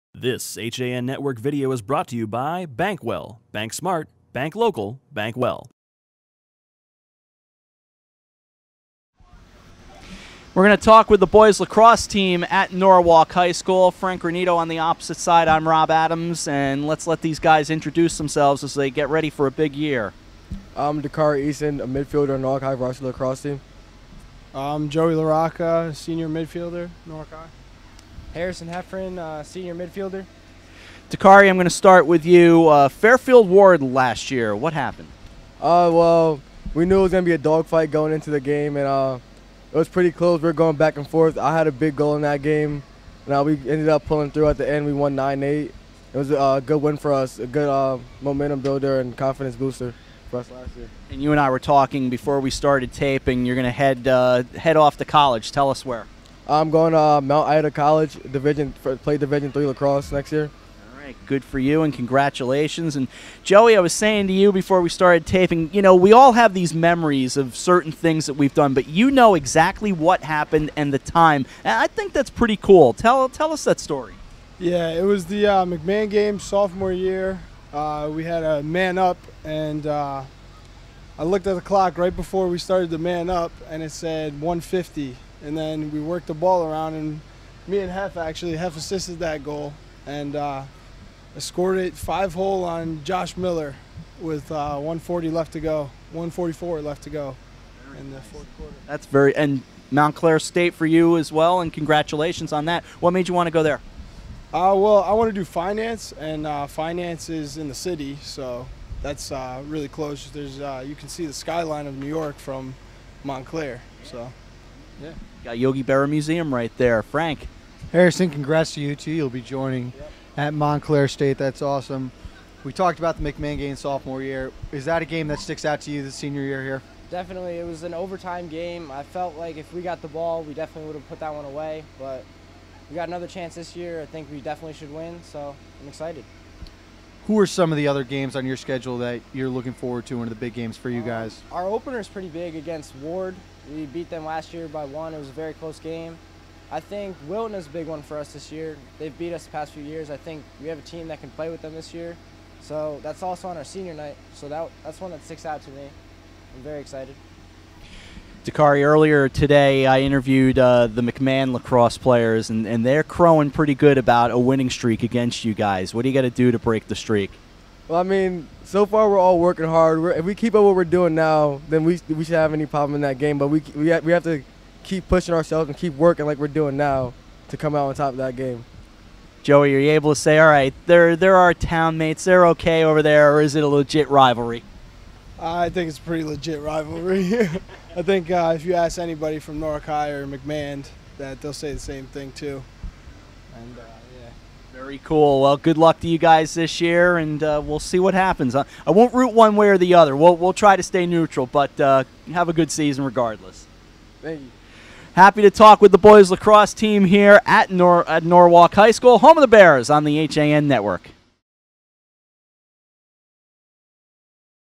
HAN FCIAC Spring Preview: Norwalk Boys Lacrosse 2016
The HAN Network stopped at Norwalk  High School on Tuesday, March 22, to interview the spring sports team captains and coaches. Here's our interview with the Bears’ boys lacrosse captains Joey Laracca, Dakari Eason and Harrison Hefferan.